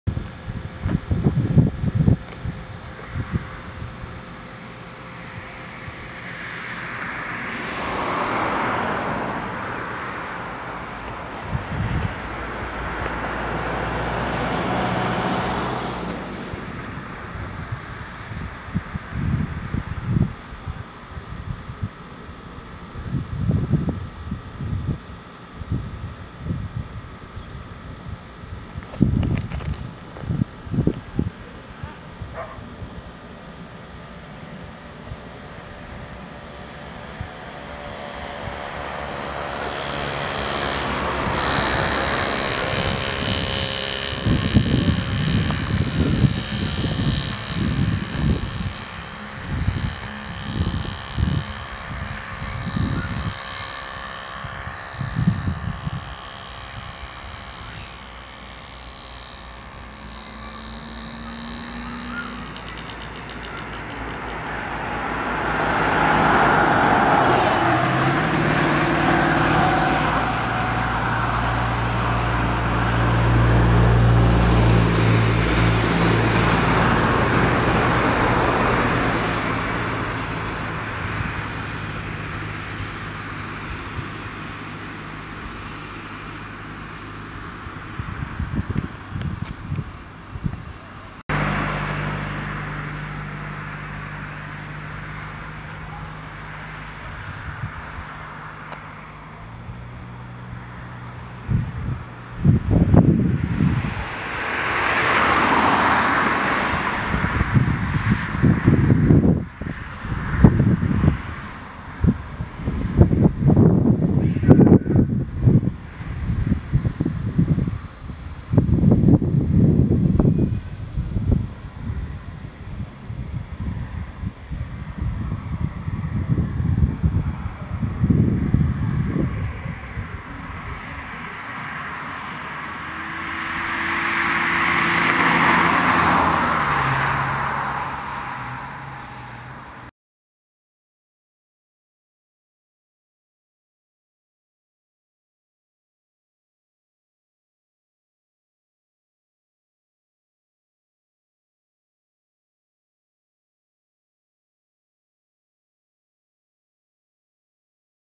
Bright Flashes of Light in the Sky without Thunder
This video was recorded on Sunday September 16th, 2012 outside my house in Paramaribo, Suriname. For at least 30 minutes there were rapid bright flashes in the sky without the sound of thunder near or in the distance. As can be seen in the video, these flashes occurred very close and lit up the entire sky. They came from behind the white cloud formation, which appeared to be too bright in the dark sky - it was night the sun was already down.

At the end of the video I recorded a sequence with a fisheye lens, enabling you to see the entire sky (180 degrees around).

Apart from traffic and the wind, it was very quiet.

Could this be lightning? Lightning doesn't usually strike rapidly and constantly like this. In addition, lightning without thunder is impossible. It was also very close, as can be seen in the video, so you'd expect to hear thunder.

If this is not lightning, then what is it?